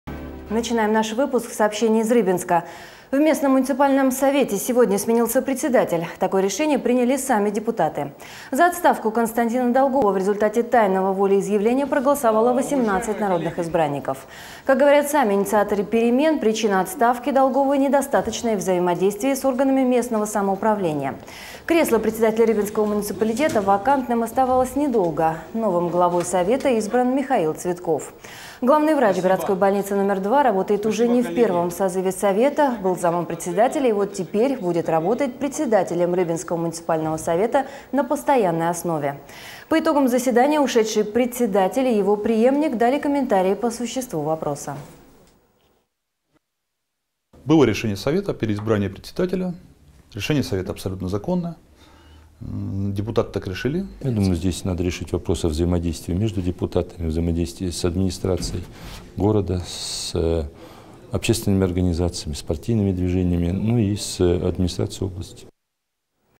В Рыбинском муниципальном совете сменился председатель
В Рыбинском муниципальном совете сегодня сменился председатель. Такое решение приняли сами депутаты. За отставку Константина Долгова в результате тайного волеизъявления проголосовало 18 народных избранников. Как говорят сами инициаторы перемен, причина отставки Долгова - недостаточное взаимодействие с органами местного самоуправления. Кресло председателя рыбинского муниципалитета вакантным оставалось недолго. Новым главой Совета избран Михаил Цветков. Главный врач городской больницы № 2 работает уже не в первом созыве Совета, был замом председателя и вот теперь будет работать председателем Рыбинского муниципального Совета на постоянной основе. По итогам заседания ушедший председатель и его преемник дали комментарии по существу вопроса.